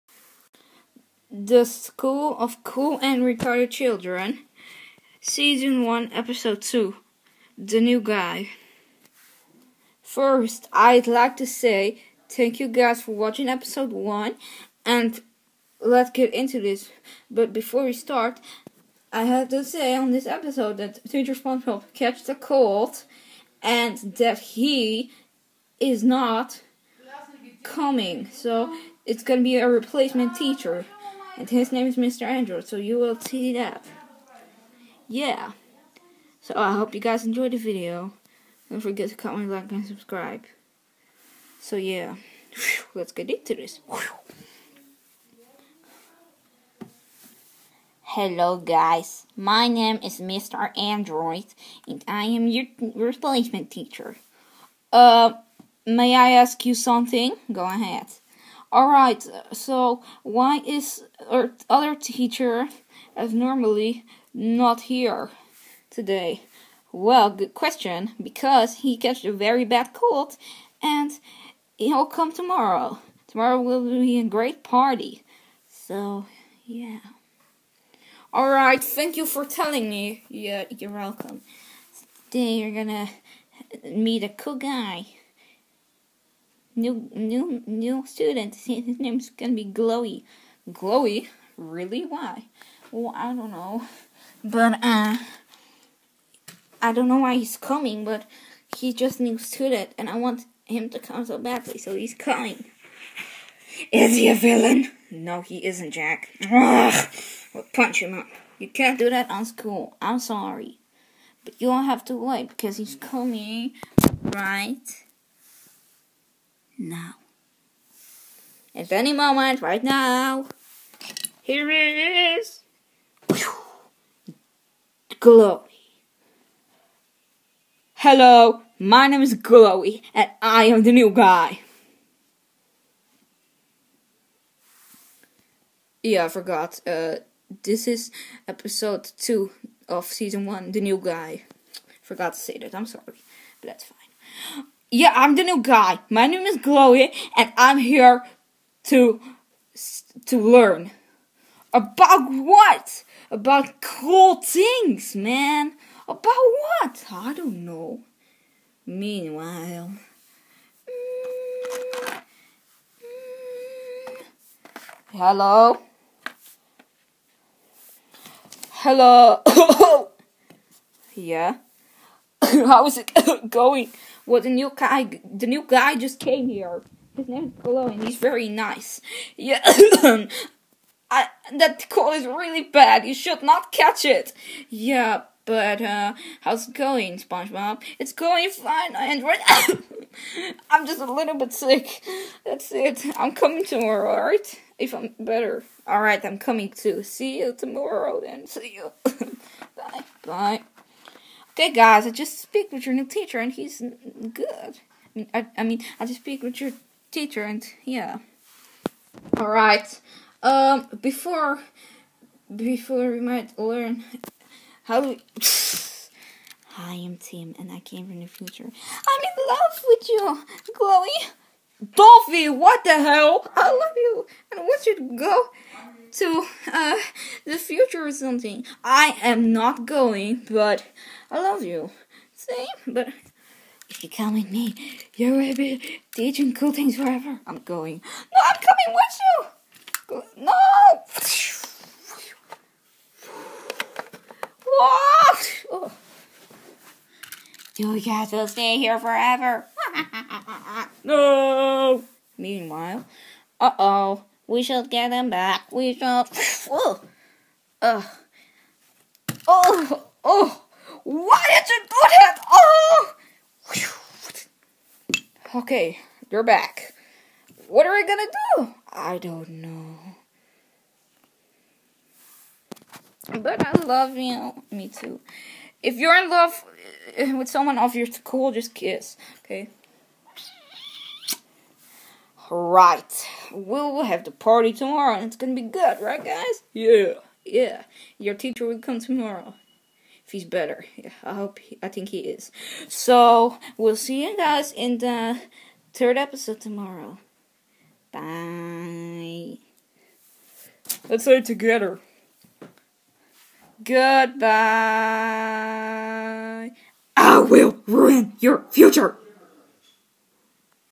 The School Of Cool & Retarded Children Season 1 Episode 2 The New Guy!
In this episode of TSOFC&RC... Teacher SpongeBob Catches A Very Bad Cold, And There's A Replacement Teacher Waiting For The Children… Do You Want To Know What Happened In This Episode? Watch And Find Out on the 2nd Episode of The School Of Cool & Retarded Children!